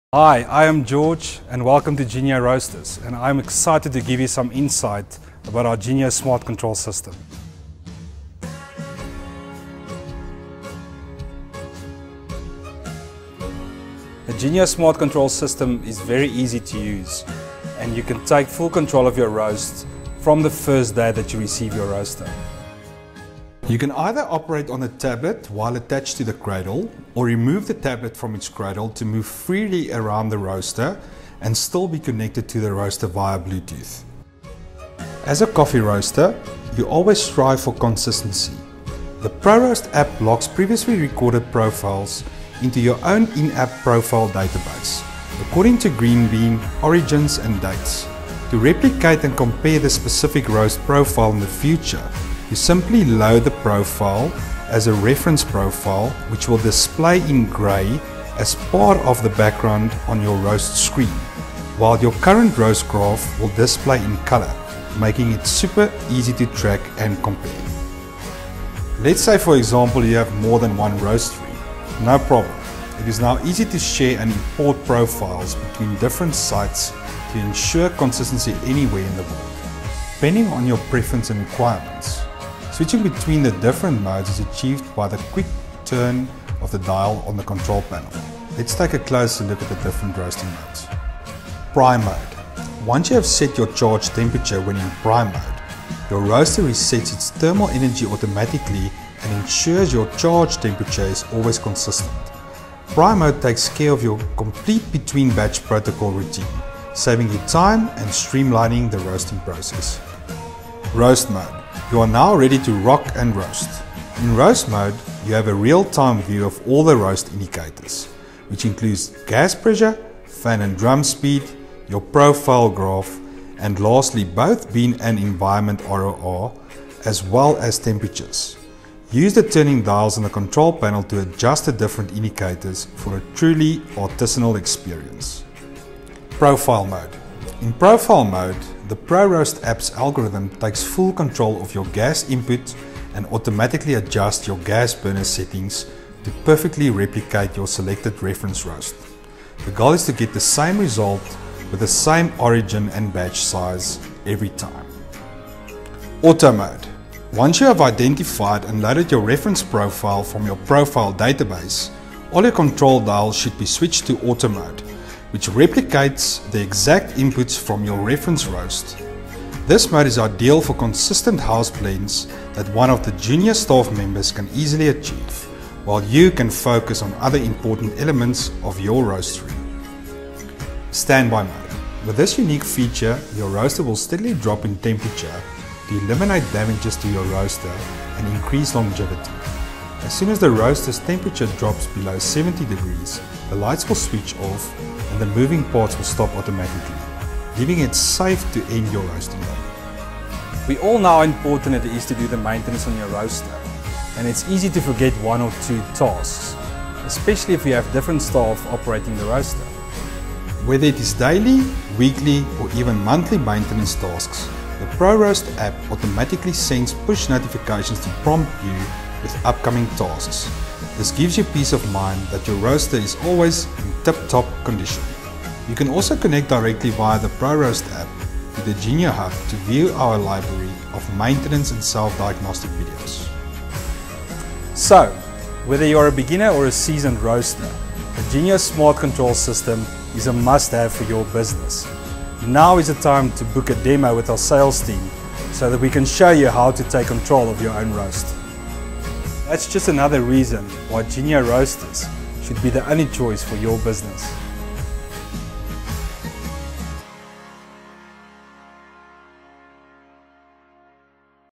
Unlock Consistency: The Genio ProRoast Control System for Specialty Coffee Roasting
Transform Your Coffee Roasting with the Genio ProRoast Control System

Discover how the Genio ProRoast Control System can revolutionize your coffee roastery business! With its advanced technology, achieving consistency and complete control over your roasting process has never been easier.

This innovative control system includes the latest Samsung Galaxy Tablet, a user-friendly control panel with turning dials, and the powerful ProRoast Android App, all included in the purchase price of your roaster—no hidden costs, subscriptions, or paid features!

Key Features:
Comprehensive Roast Management: The ProRoast App allows operators to log, load, and manage roast profiles on compatible Genio coffee roasting machines.
Real-Time Monitoring: Track crucial roast parameters during each batch, including Bean Temperature, Environment Temperature, Bean Rate-of-Rise, Environment Rate-of-Rise, Gas Settings, Fan Speed, and Drum Speed Settings.
Profile Storage and Retrieval: Store your unique roast profiles in the system and easily load them for future reference, ensuring consistency across batches.
Origin Tracking: Effortlessly associate roast profiles with specific green bean origins, enhancing your ability to deliver tailored flavor profiles to your customers.
Join us as we dive into the features and benefits of the Genio ProRoast Control System, designed to elevate your roasting experience and empower you to create exceptional coffee every time.